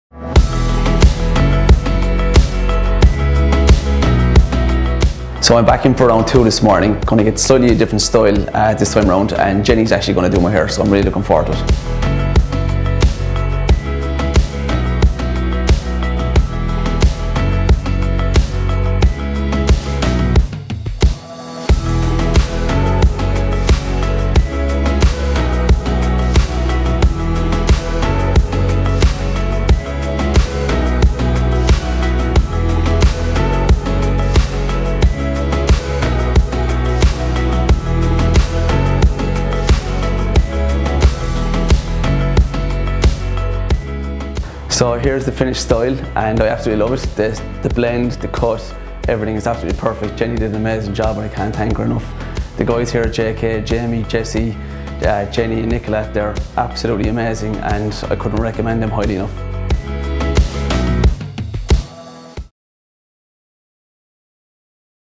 JK Hair Replacement System transformation - to a full head of hair!
Our stylist Jenny transforms our client Derek to a full head of natural hair! Building confidence and making dreams come true! Derek is delighted with this system, which is his second system with us (see his first transformation in our other video) Get your hair back today, the JK way!